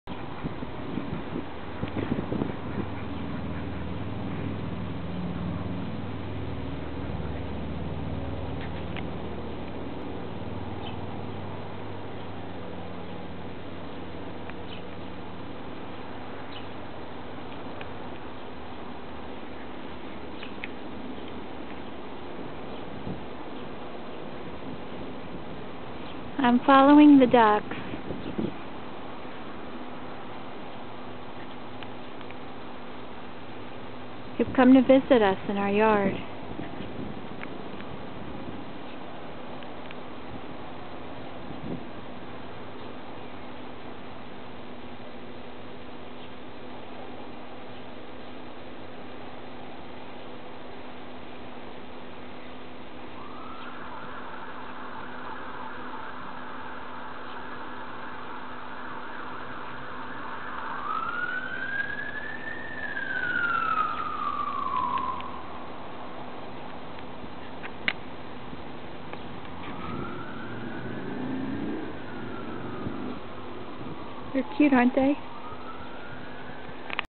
Visiting ducks again this year.
I guess the word is out that the bird feed has spilled over.  The ducks have returned!

Quack Quack!